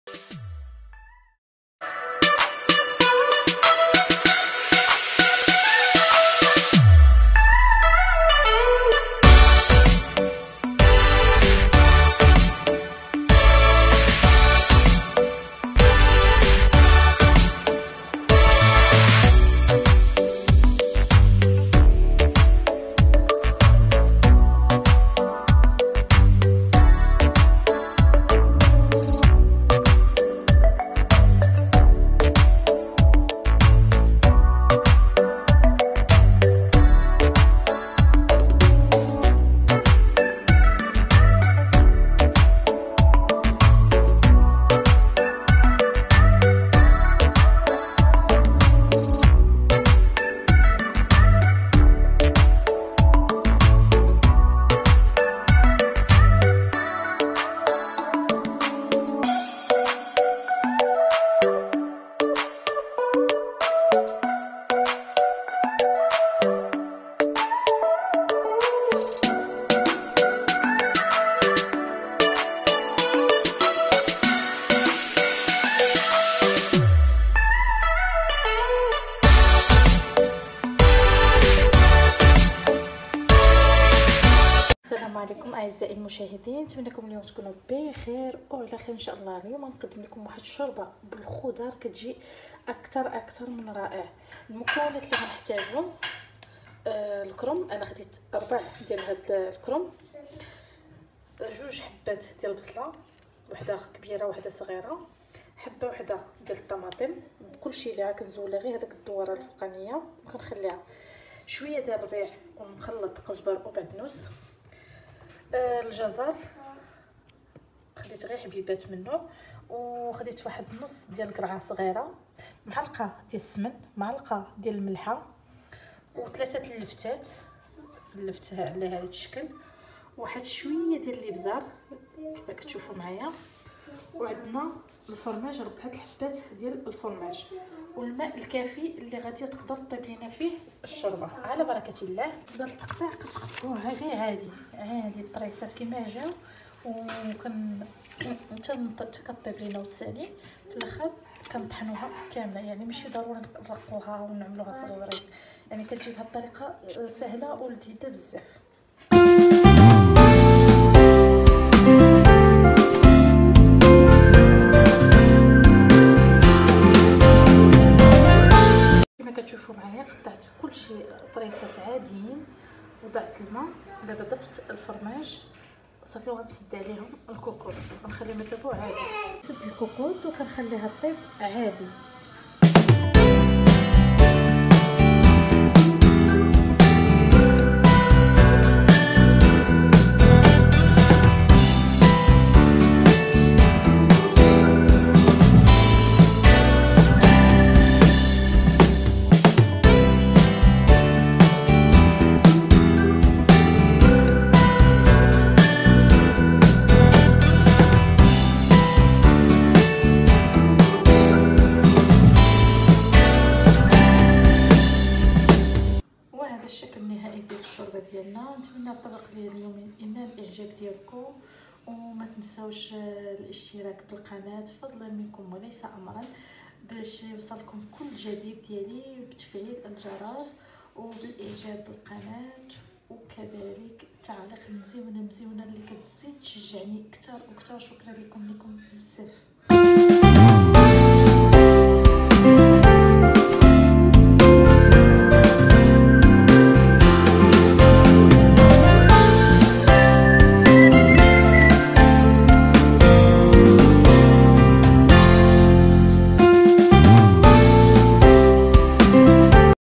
طريقة تحضير شوربة بالخضر اكثر من رائعة
المكونات
البصل
القرعة البرتقالية
اللفت
الجزر
الربيع
الكرم
الملح  الفرماج مثلث
السمن
الماء الكافي للطهي
ملاحظة يمكن اضافة اي نوع من الخضر موجودة